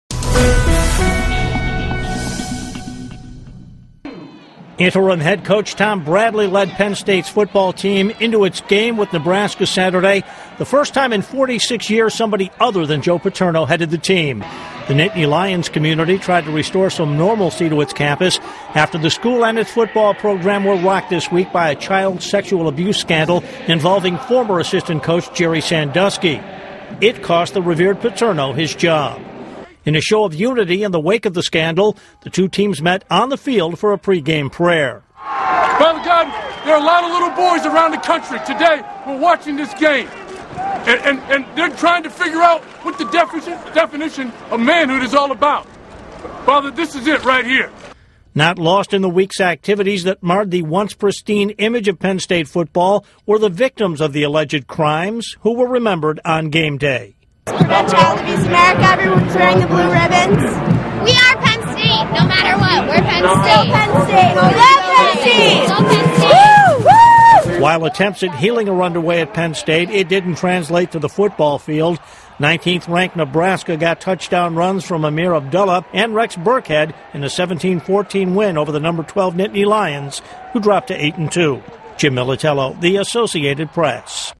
Penn State Falls in Return to Football
Penn State loses to Nebraska at the end of a week in which the child sexual abuse scandal tarnished the program and cost it's long time coach his job. (Nov. 12)